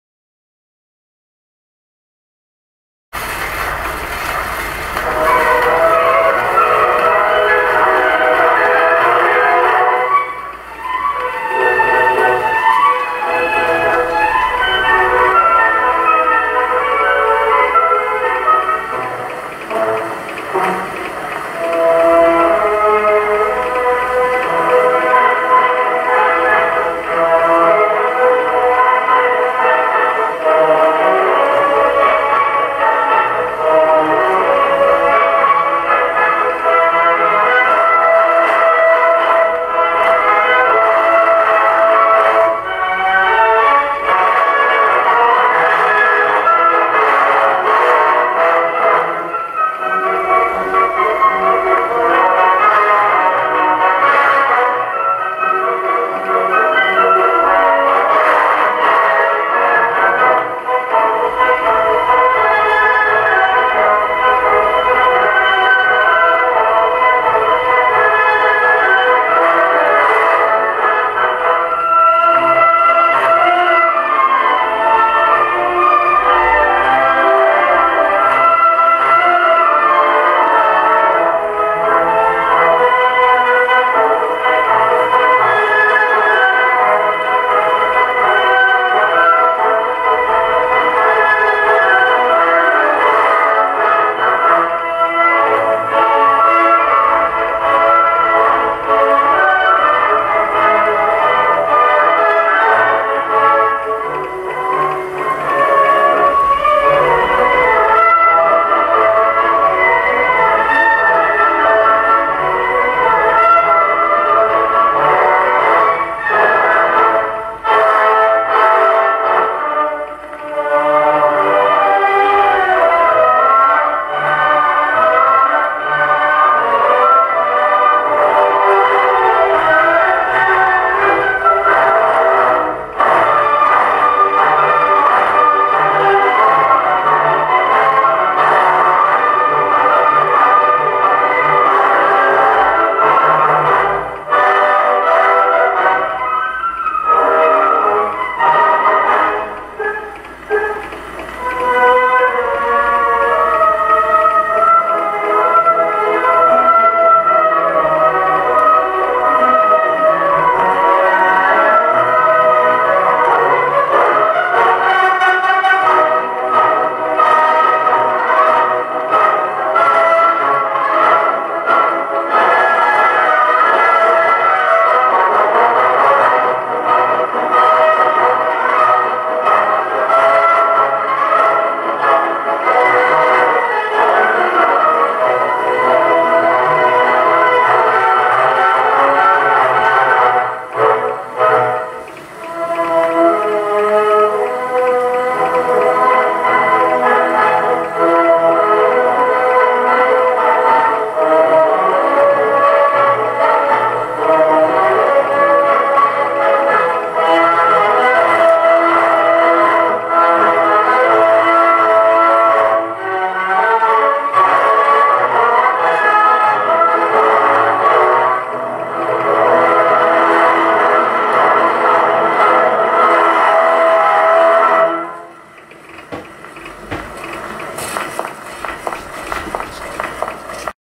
Cheney Hepplewhite Gramophone
This is a gramophone made by Cheney Talking Machine Company. The model of the gramophone is style 3 Hepplewhite. Serial No. 4C332.